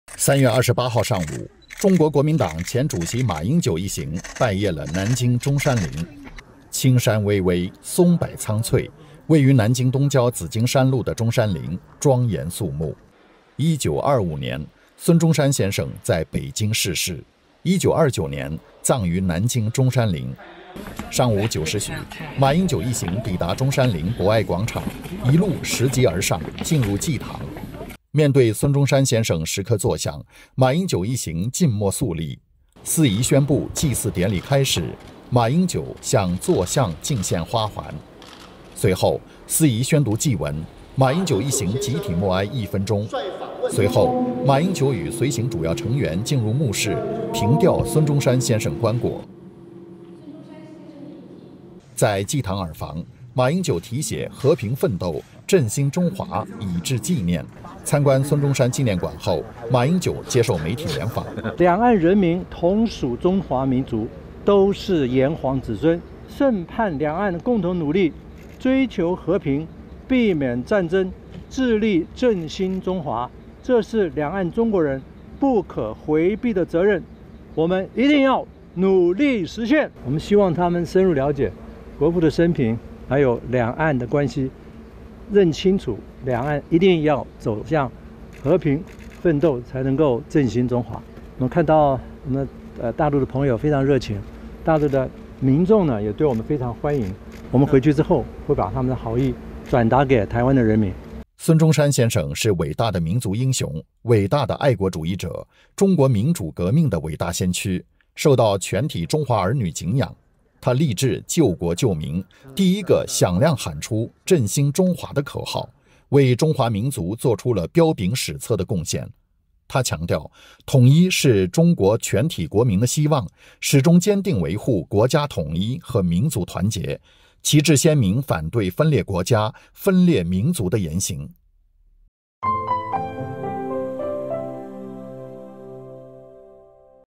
马英九拜谒南京中山陵 题写“和平奋斗 振兴中华”
3月28日上午，中国国民党前主席马英九一行拜谒了南京中山陵。

青山巍巍，松柏苍翠，位于南京东郊紫金山麓的中山陵庄严肃穆。1925年，孙中山先生在北京逝世，1929年葬于南京中山陵。

上午9时许，马英九一行抵达中山陵博爱广场，一路拾级而上，进入祭堂。面对孙中山先生石刻坐像，马英九一行静默肃立。司仪宣布祭祀典礼开始，马英九向坐像敬献花环。

随后，司仪宣读祭文，马英九一行集体默哀一分钟。随后，马英九与随行主要成员进入墓室，凭吊孙中山先生棺椁。

在祭堂耳房，马英九题写“和平奋斗 振兴中华”，以志纪念。参观孙中山纪念馆后，马英九接受媒体联访。

孙中山先生是伟大的民族英雄、伟大的爱国主义者、中国民主革命的伟大先驱，受到全体中华儿女景仰。他立志救国救民，第一个响亮喊出“振兴中华”的口号，为中华民族作出了彪炳史册的贡献。他强调“‘统一’是中国全体国民的希望”，始终坚定维护国家统一和民族团结，旗帜鲜明反对分裂国家、分裂民族的言行。（路梅 赵安源）